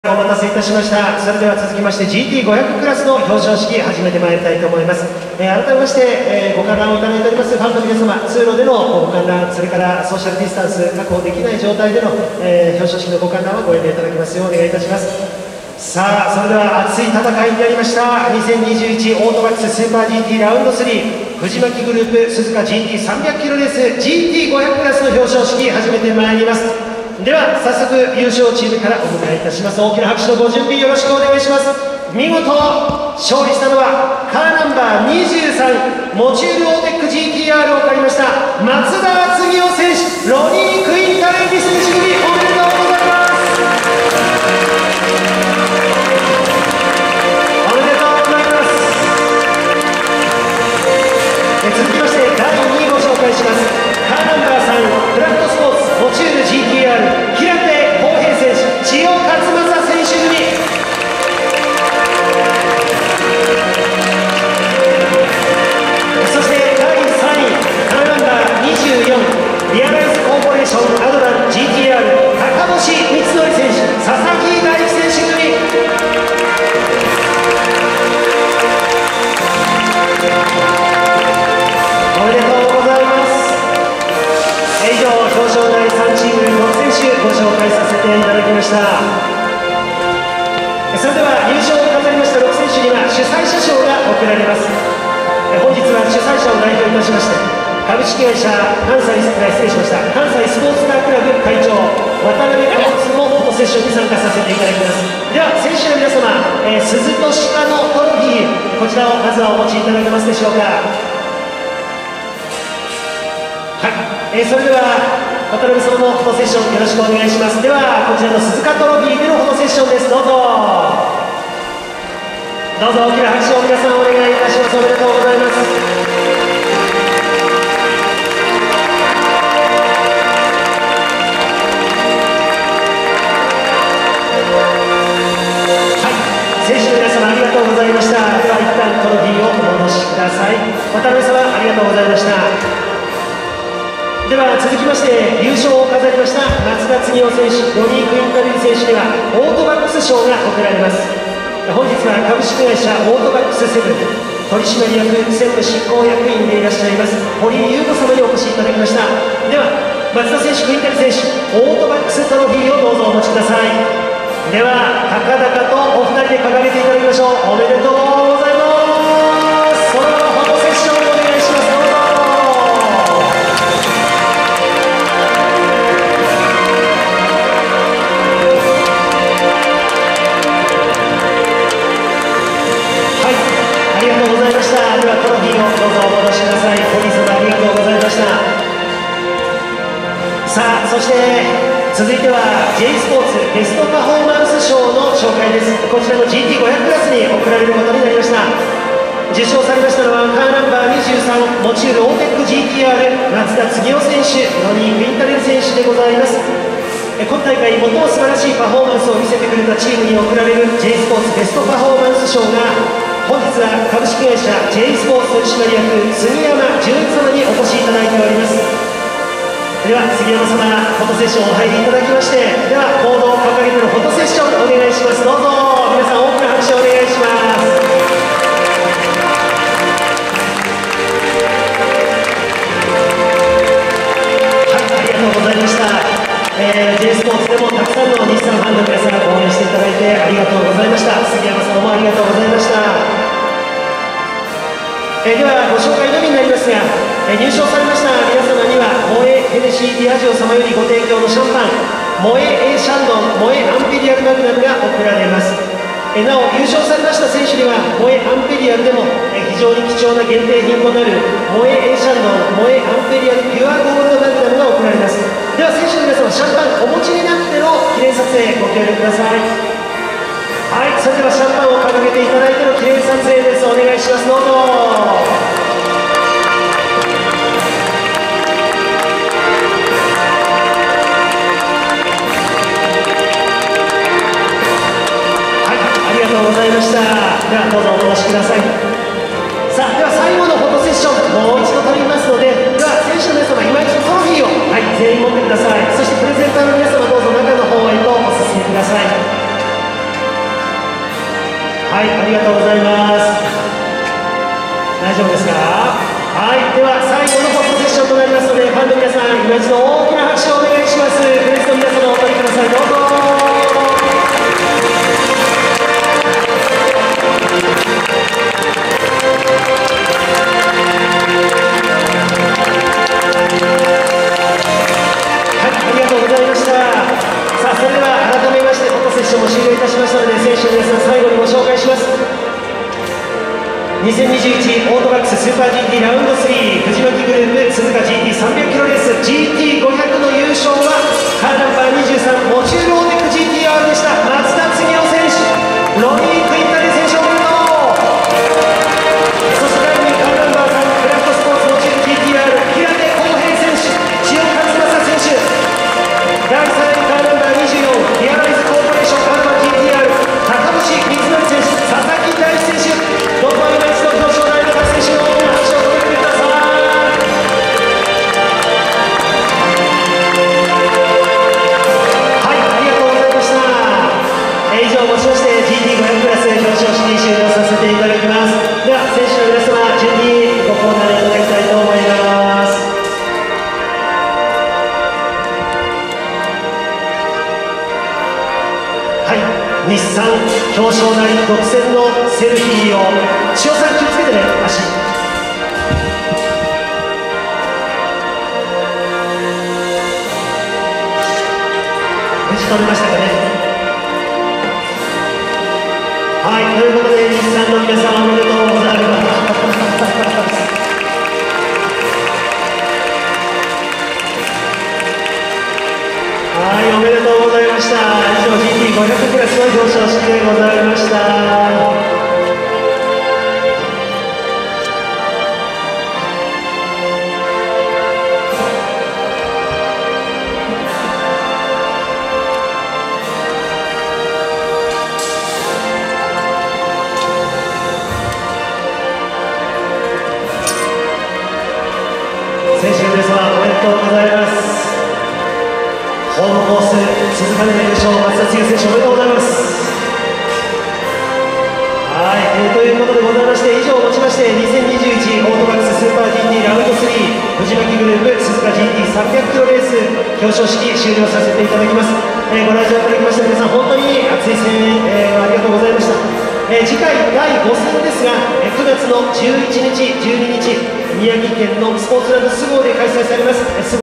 GT-R完全勝利！ 表彰台独占！ 2021 スーパーGT 鈴鹿 Rd.3 表彰式
2021/08/22 鈴鹿サーキット
AUTOBACS SUPER GT Rd.3 FUJIMAKI GROUP SUZUKA GT 300km RACE